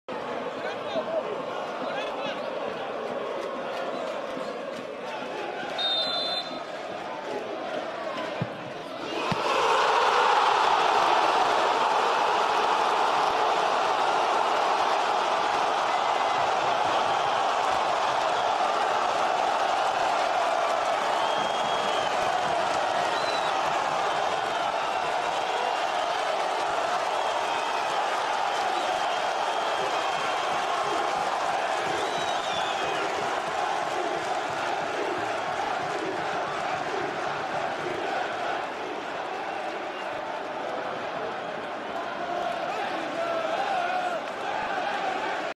Goal | Golo Grimaldo: Paços de Ferreira 0-(1) Benfica (Liga 22/23 #20)
Grande golo de Grimaldo! Livre batido bem ao estilo do espanhol, abola passa por cima da barreira e entra junto ao poste. Marafona bem se lançou, ainda tocou mas não conseguiu evitar o 1-0!